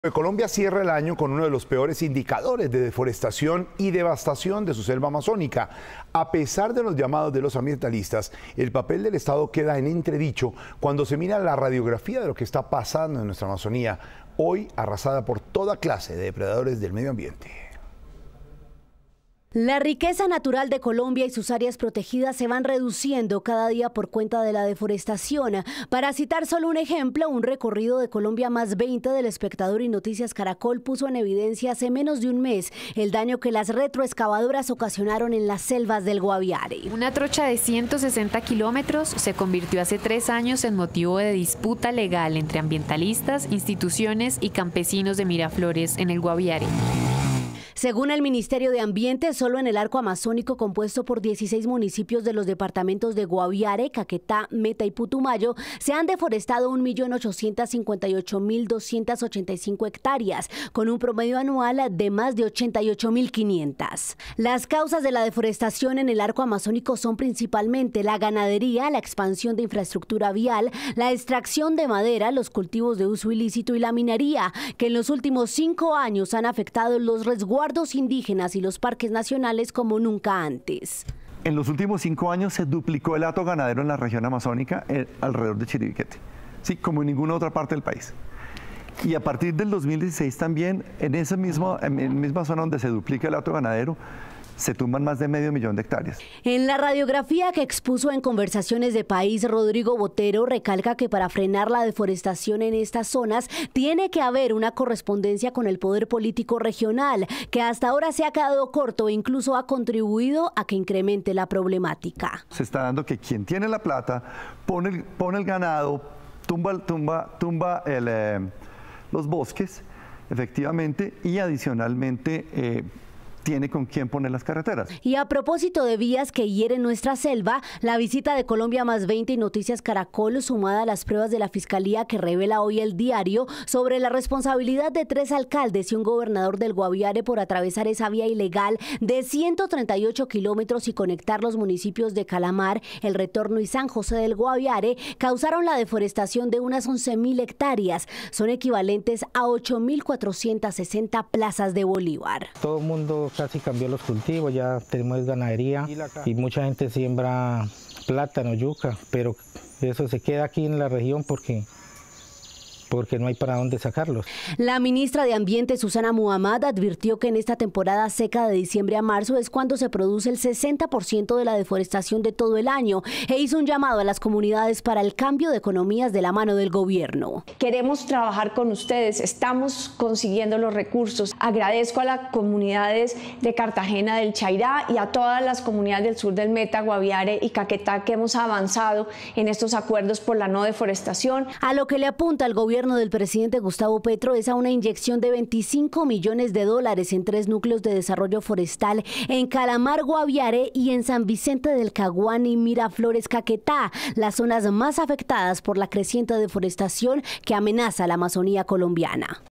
Colombia cierra el 2022 con graves indicadores sobre deforestación en la selva amazónica
Colombia cerró el 2022 con uno de los peores indicadores de deforestación y devastación de la selva amazónica. A pesar de los llamados de los ambientalistas, el papel del Estado quedó en entredicho cuando se analiza la radiografía de lo que está pasando en el pulmón del mundo.